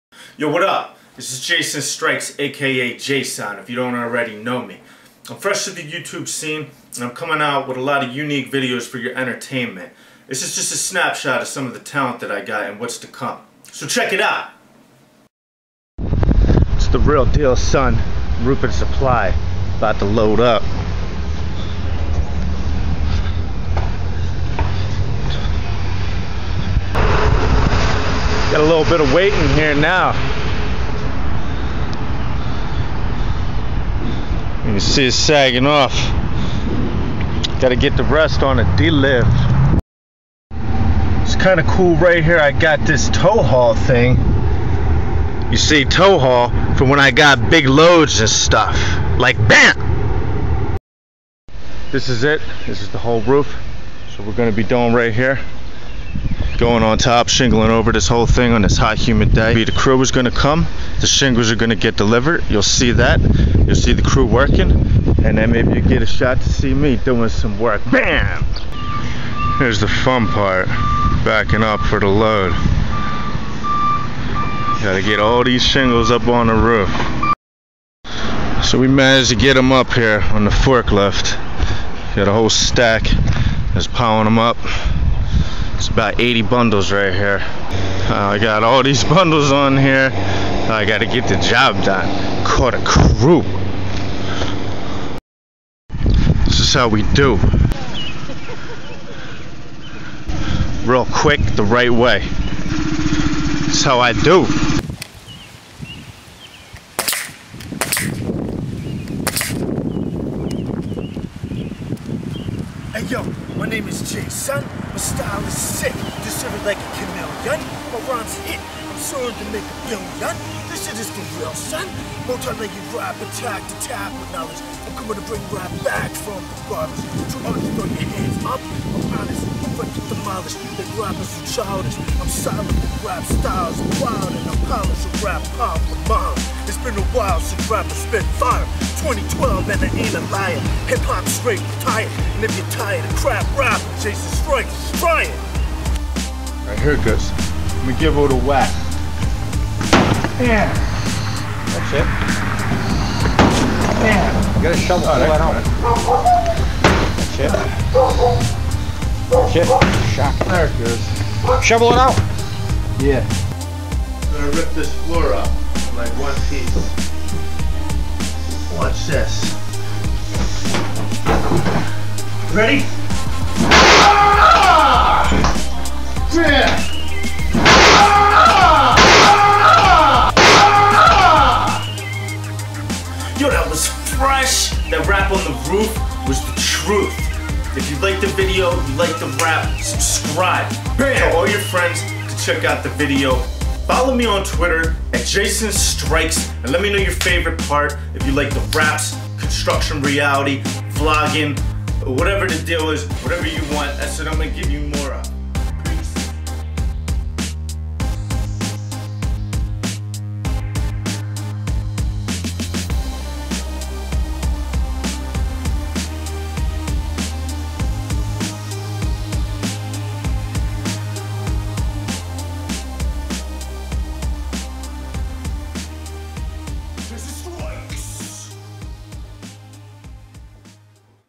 DUDE RAPS ON ROOF
JasonStrikes 1st youtube video. Contractor raps. Variety of entertainment, rap, comedic vlog, construction reality and demolition.   Music By The Passion HiFi